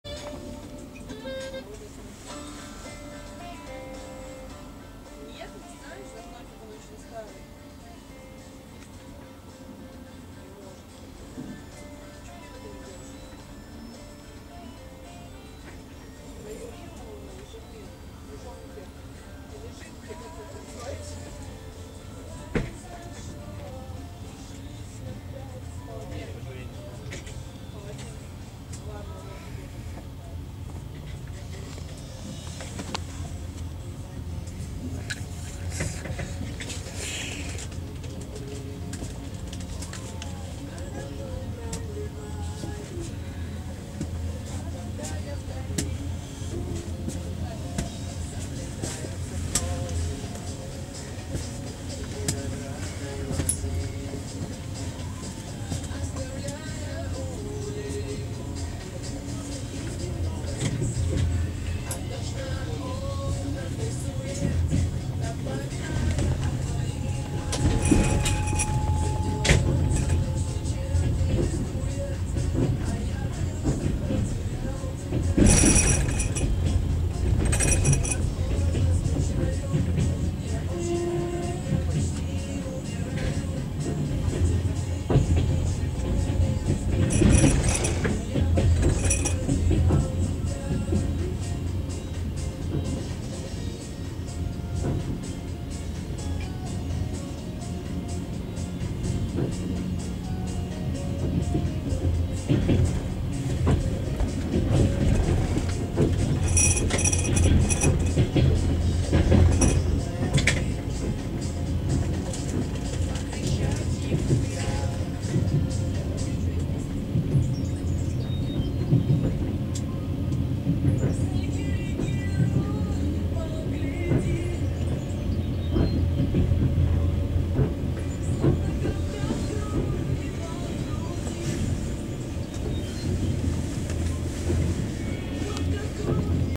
Отправление со станции Фалёнки из окна поезда
Отправление со станции Фалёнки из окна поезда № 132 Санкт Петербург - Ижевск
Россия/Кировская область
4.11.22